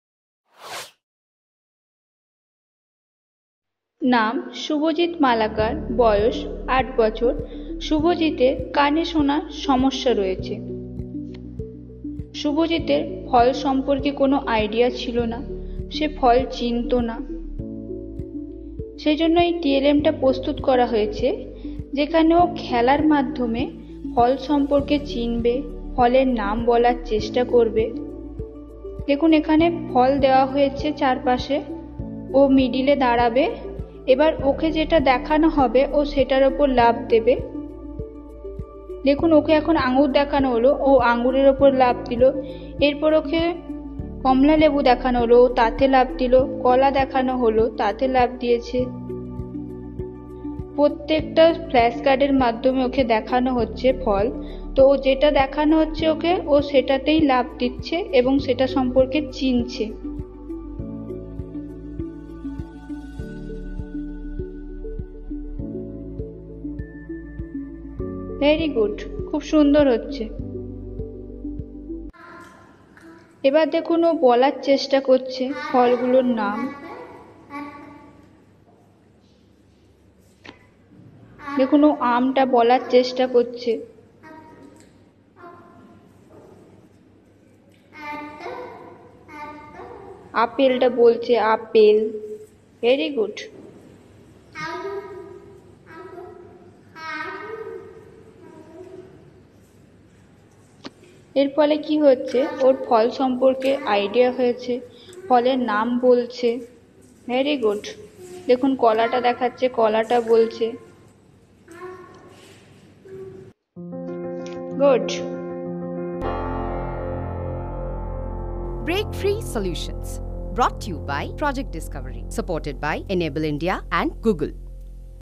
Fun Activity for Kids to identify different types of fruits. | BreakFreeSolutions
"In this solution video, Shubhojit Malakar a person with hard of hearing could not identify or tell the name of any fruits, so a circular teaching aid is made. With the help of flashcards, he could now identify different fruits like orange grapes, apples, etc. This activity is carried out through jumping activity in different flashcards of fruits. After that, he’s trained to tell the names of different fruits with the help of flash cards.
This solution video is shared by Kendua dihi Bikash Society
Edited and Presented by eilabs and Project Discovery